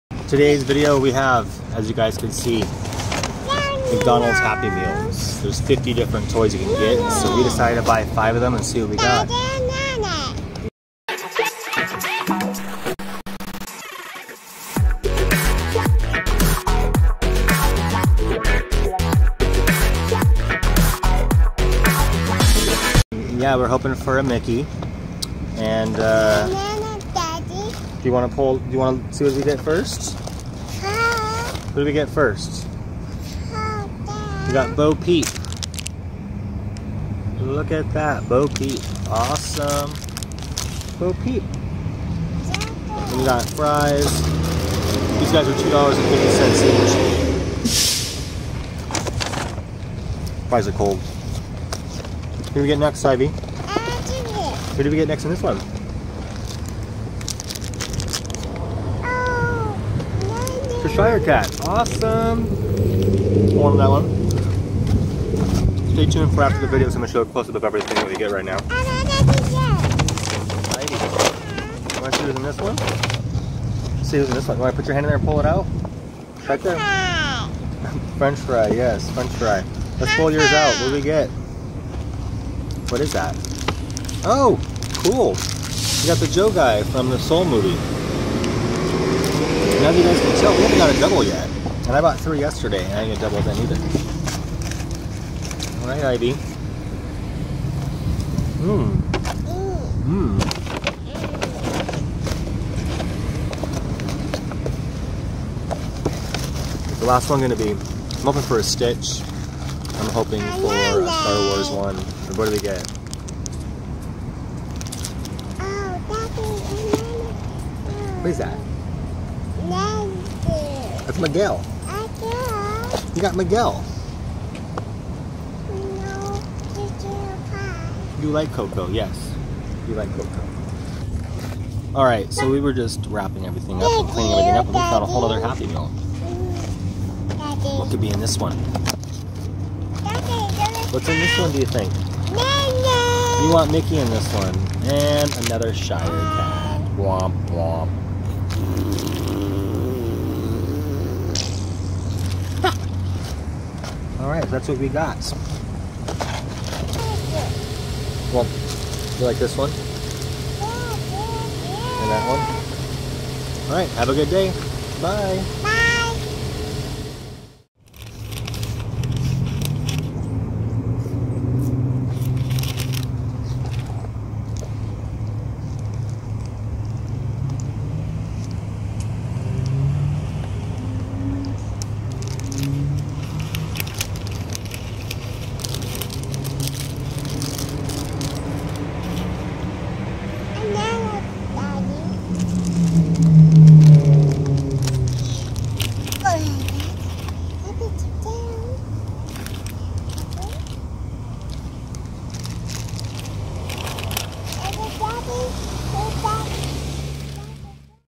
McDonald's Walt Disney World 50th Anniversary Happy Meal Toys
We got 5 happy meals..what toys did we get?